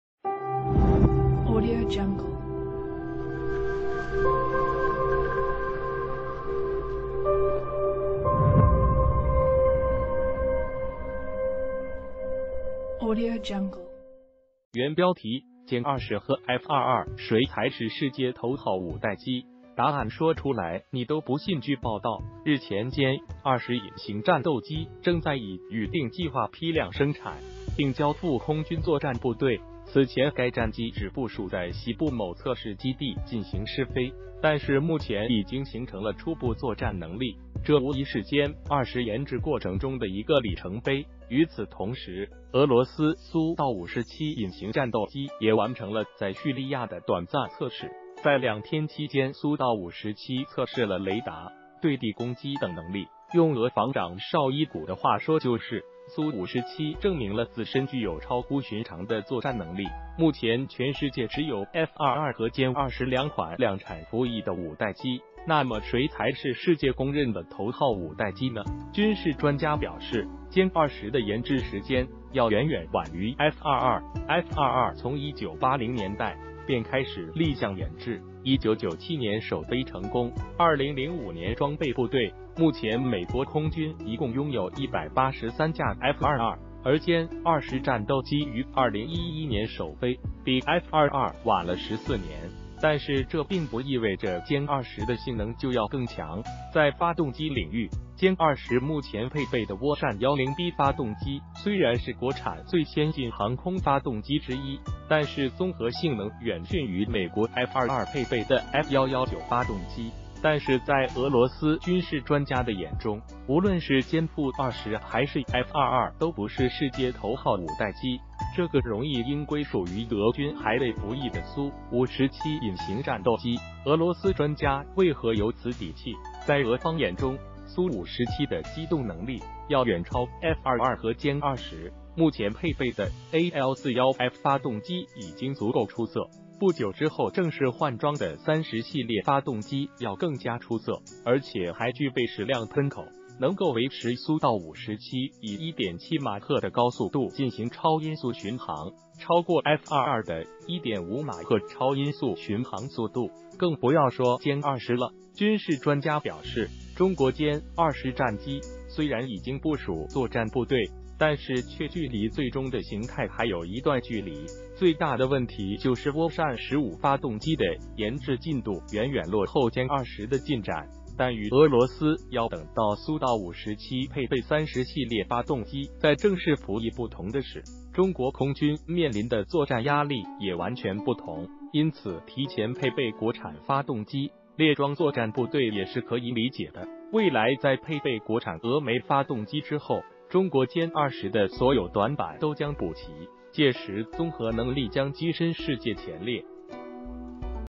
歼20和F22谁才是世界头号五代机？答案说出来你都不信
原标题：歼20和F22谁才是世界头号五代机？答案说出来你都不信据报道，日前歼-20隐形战斗机正在以预定计划批量生产并交付空军作战部队，此前该战机只部署在西部某测试基地进行试飞，但是目前已经形成了初步作战能力，这无疑是歼-20研制过程中的一个里程碑，与此同时，俄罗斯苏-57隐形战斗机也完成了在叙利亚的短暂测试，在两天期间苏-57测试了雷达、对地攻击等能力，用俄防长绍伊古的话说就是：“苏-57证明了自身具有超乎寻常的作战能力。”目前全世界只有F-22和歼-20两款量产服役的五代机，那么谁才是世界公认的头号五代机呢？军事专家表示，歼-20的研制时间要远远晚于F-22，F-22从1980年代便开始立项研制，1997年首飞成功，2005年装备部队，目前美国空军一共拥有183架F-22。而歼-20战斗机于2011年首飞，比F-22晚了14年，但是这并不意味着歼-20的性能就要更强，在发动机领域，歼-20目前配备的涡扇-10B发动机虽然是国产最先进航空发动机之一，但是综合性能远逊于美国F-22配备的F-119发动机。但是在俄罗斯军事专家的眼中，无论是歼-20还是F-22都不是世界头号五代机，这个容易应归属于俄军还未服役的苏-57隐形战斗机，俄罗斯专家为何有此底气？在俄方眼中，苏-57的机动能力要远超F-22和歼-20，目前配备的AL-41F发动机已经足够出色，不久之后正式换装的“30系列”发动机要更加出色，而且还具备矢量喷口，能够维持苏-57以1.7马赫的高速度进行超音速巡航，超过F-22的1.5马赫超音速巡航速度，更不要说歼-20了。军事专家表示，中国歼-20战机虽然已经部署作战部队，但是却距离最终的形态还有一段距离，最大的问题就是涡扇-15发动机的研制进度远远落后歼-20的进展，但与俄罗斯要等到苏-57配备30系列发动机再正式服役不同的是，中国空军面临的作战压力也完全不同，因此提前配备国产发动机列装作战部队也是可以理解的。未来在配备国产“峨眉”发动机之后，中国歼-20的所有短板都将补齐，届时综合能力将跻身世界前列。